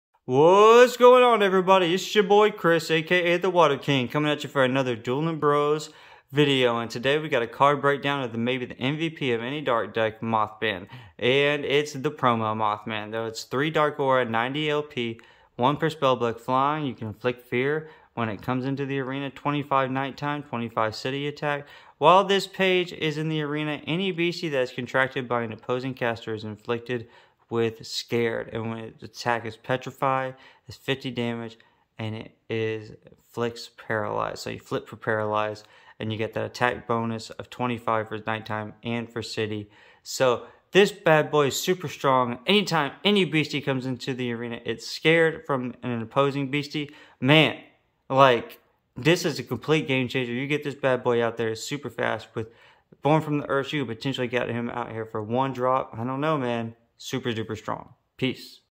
🌃Card Breakdown: C.N. Mothman Promo🌙
Today's card breakdown features in my opinion the most well rounded of the 3 different mothman. Its Arena effect can shut down your opponent pretty fast and it's only a 3 cost!

Hope you guys are having a great week! Peace Dueling Bros Fam!! 😇🙏💯❤